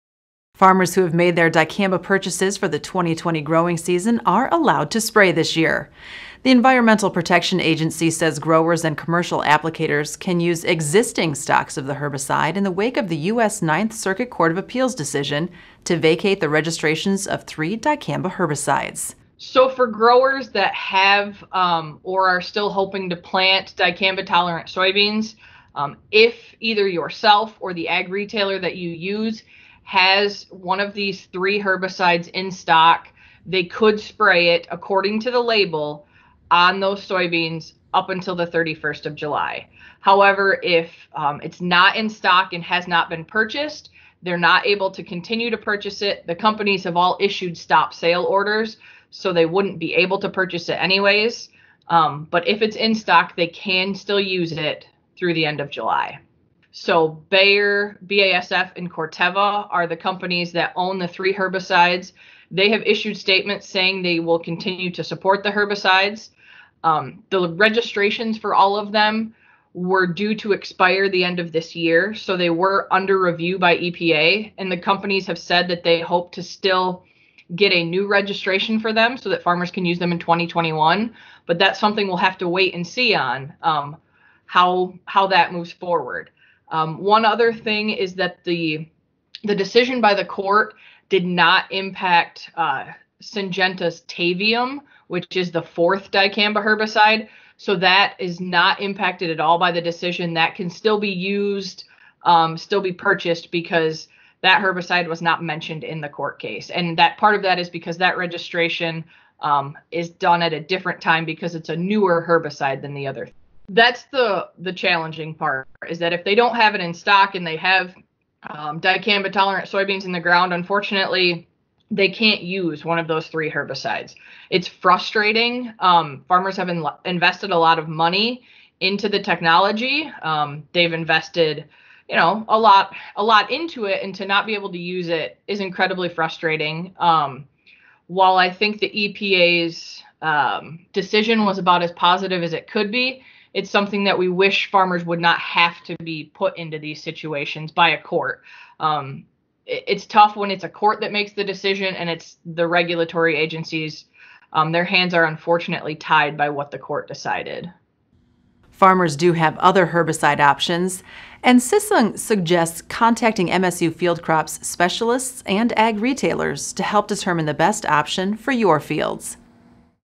FN5 Daily: EPA says farmers can use existing dicamba stocks
Farmers who have made their Dicamba purchases for the 2020 growing season are allowed to spray this year. The Environmental Protection Agency says growers and commercial applicators can use existing stocks of the herbicide, in the wake of the U.S. Ninth Circuit Court of Appeals decision to vacate the registrations of three dicamba herbicides.